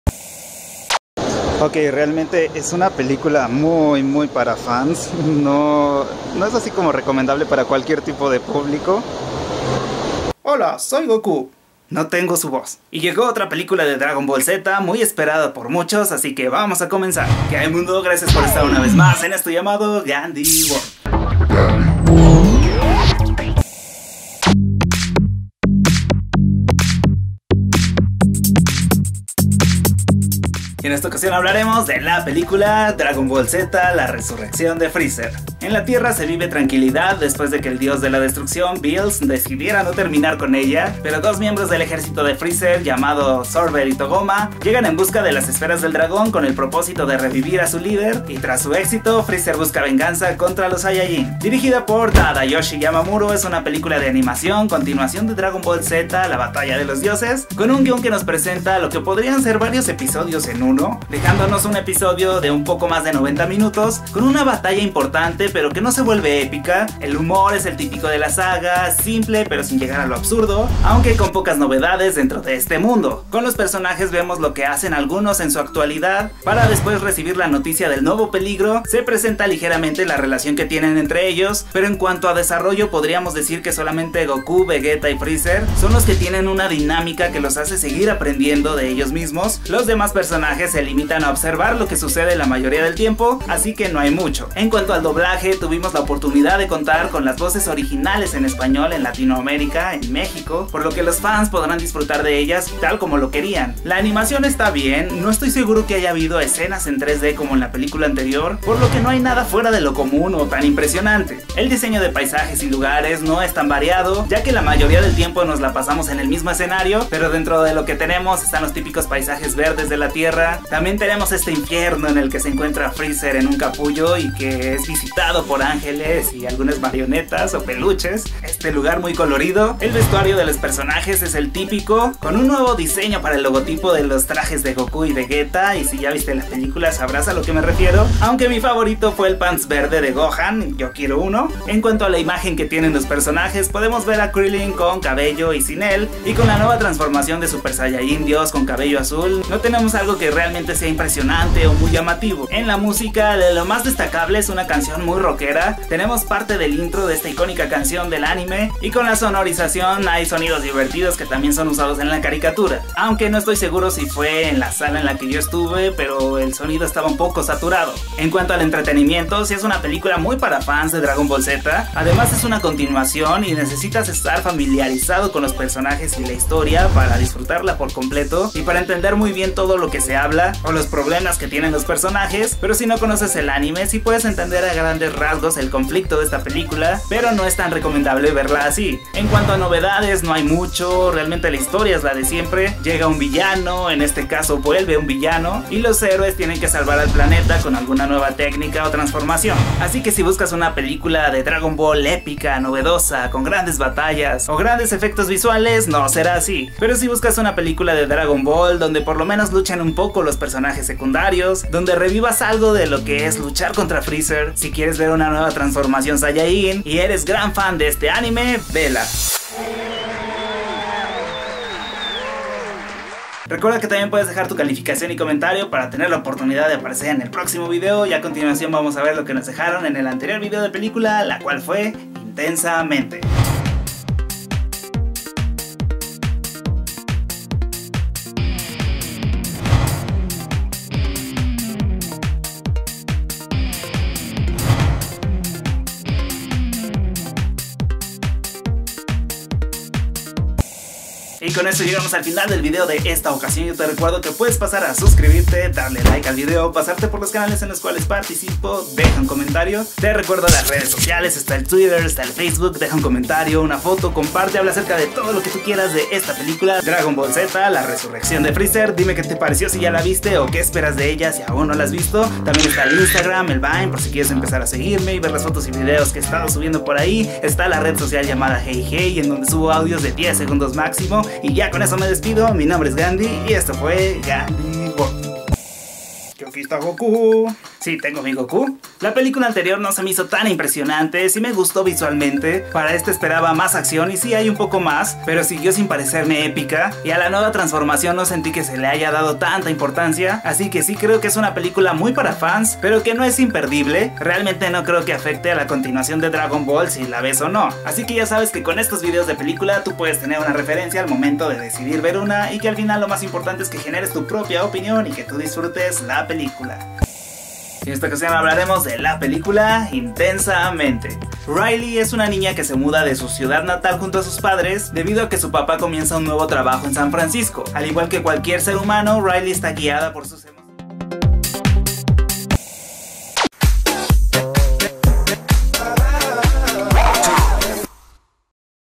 Dragon Ball Z: La resurrección de Freezer (Opinión)
Crítica de la película "Dragon Ball Z: La resurrección de Freezer" dirigida por Tadayoshi Yamamuro.

Vine: Gandhiworld